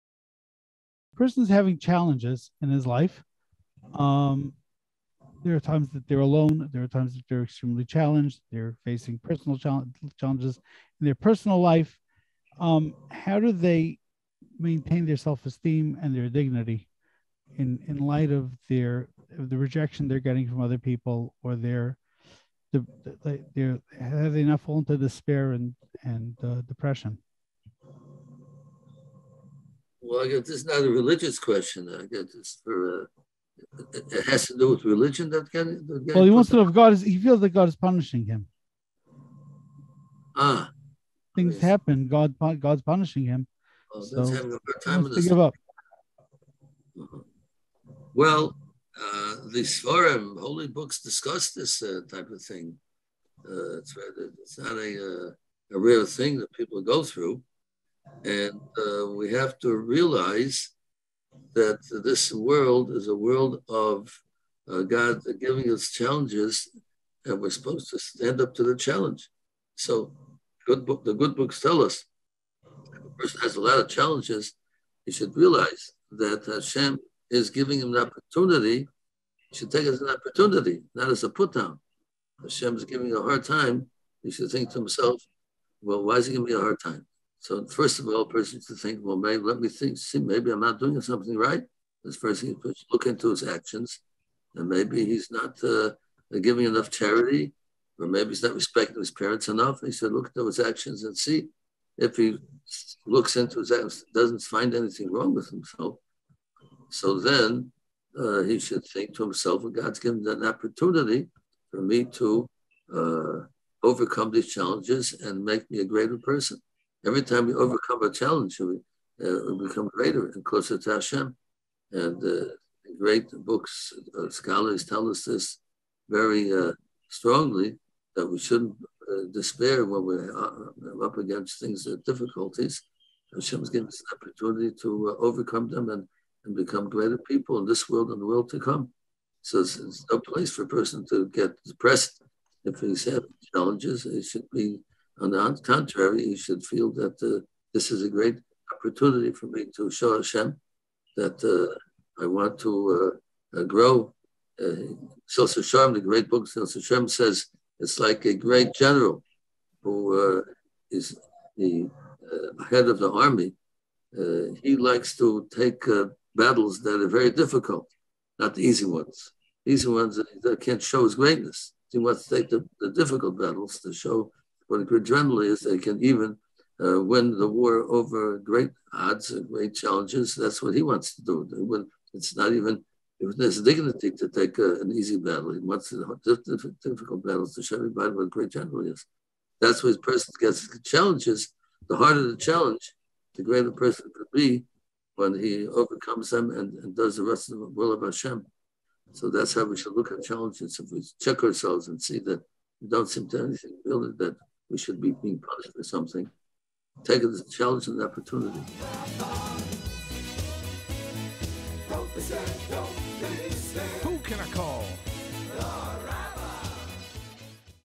How do I maintain my dignity in face of challenge? | Ask the Rabbi Live with Rabbi Chaim Mintz
This is a world full of challenges and we must stand up to them. First, look into your actions to see if there's any wrong. Then, realize that G-d is challenging you to make you greater and bring you closer to Him. G-d only gives these opportunities to those who are up to it,  so don't despair - you are a great person.